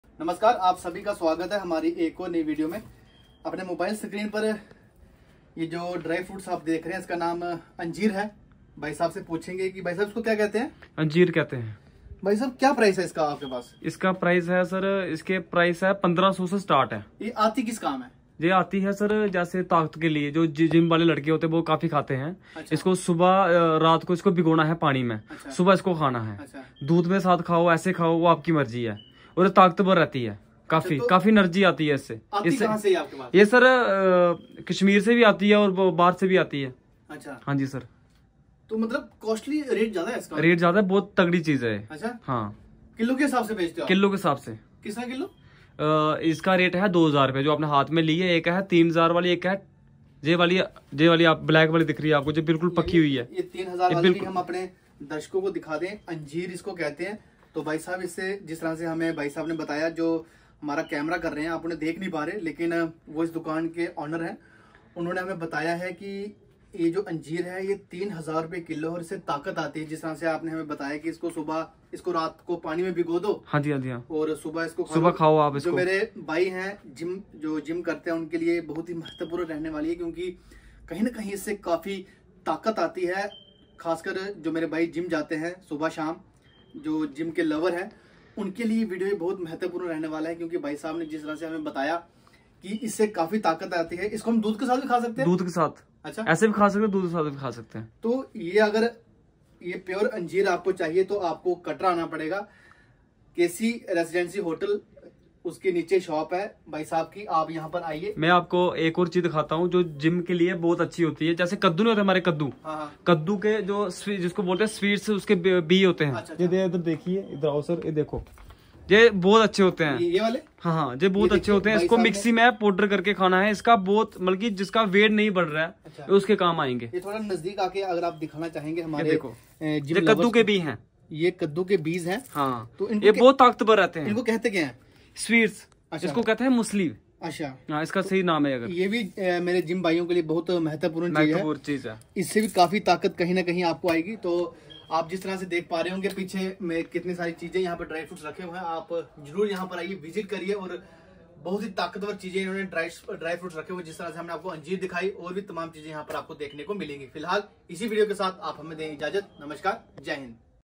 3000 रुपए किलो यह ड्राई फ्रूट्स अंजीर आपको देगा काफी ताकत
3000 रुपए किलो यह ड्राई फ्रूट्स अंजीर आपको देगा काफी ताकत